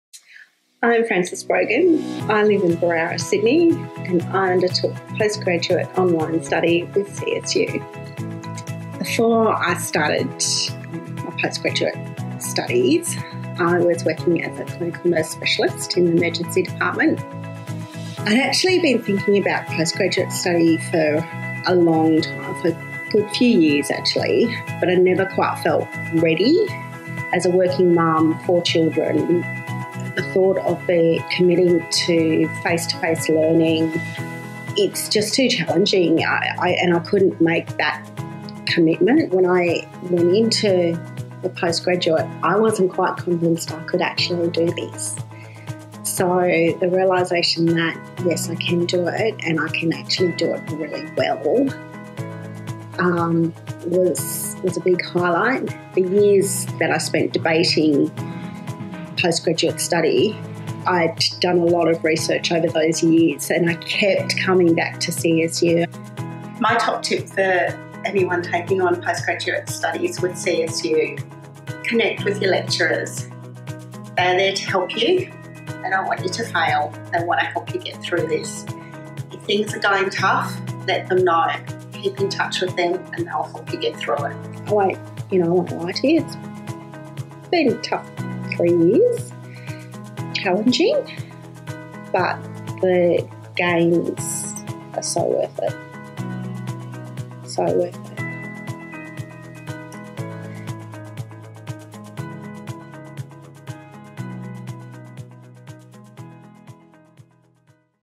Making her next move count: how Frances advanced her career at Charles Sturt
Frances Brogan was keen to progress her career in nursing – and move away from shiftwork so she could spend more time with her family. Studying a Master of Nursing online with Charles Sturt University helped her achieve her goals.